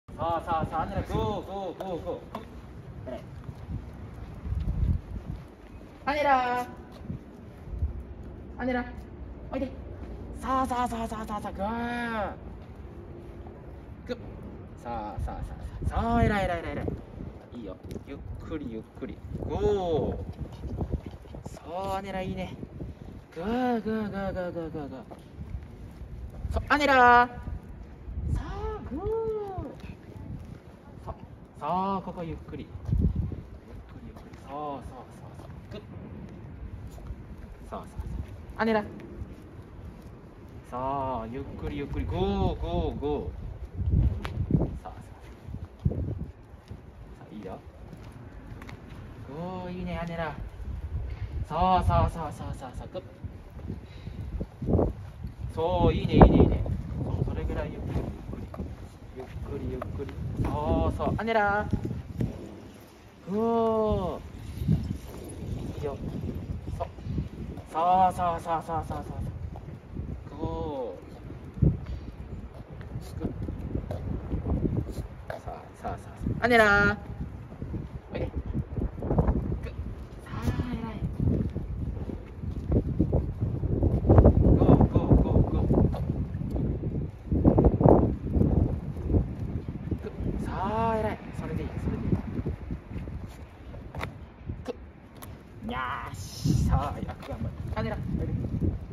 ポメラニアンの”アネラちゃん”と散歩トレーニング！
ポメラニアンの”アネラちゃん”と散歩トレーニングをしています！
普段のお散歩でいろんな方向に引っ張るので、落ち着いて人の隣を歩けるように練習☆
初めは左側でしっかりとリードを持って、アイコンタクトを取りながら隣を歩く事が出来たら褒めています♪練習を重ねると、リードが少し緩んだ状態でもしっかりと隣を歩く事ができるようになりました(^_^)